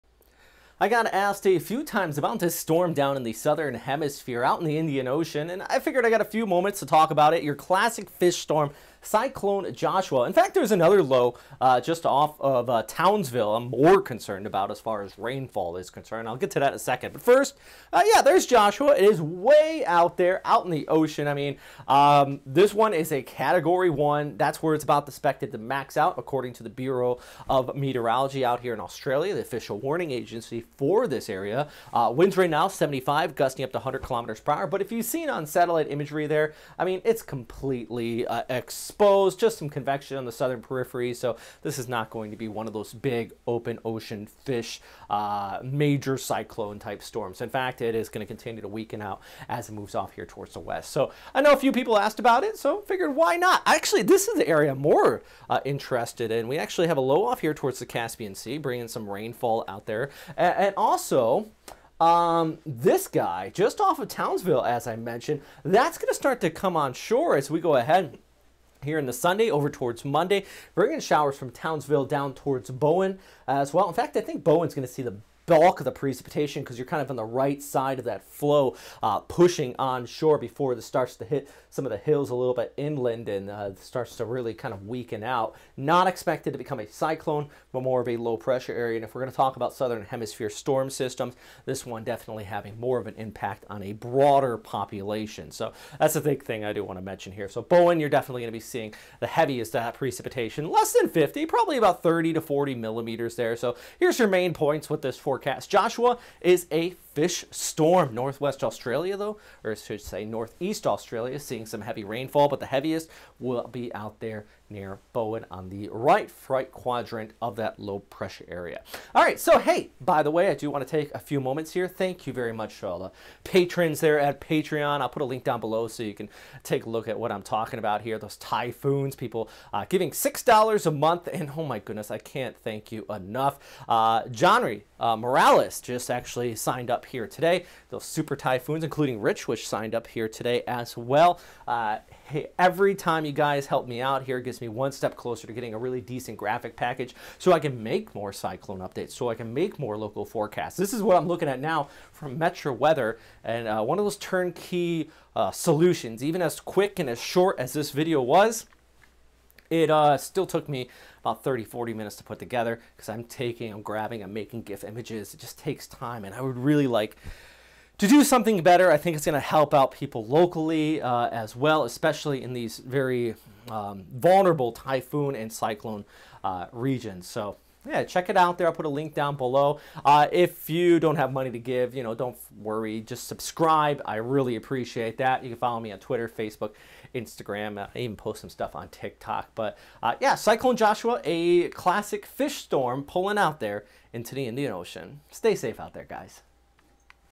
Cyclone Joshua and Coral Sea Low (17th Jan. 2021)
I was asked a few times about this new cyclone in the Indian Ocean, he is a classic fish storm but a cyclone none the less so we can touch on it real quick.

Meanwhile in the Coral Sea a Low Is going to bring some rains from Townsville to Bowen. As far as tropical impacts I think this one is more noteworthy.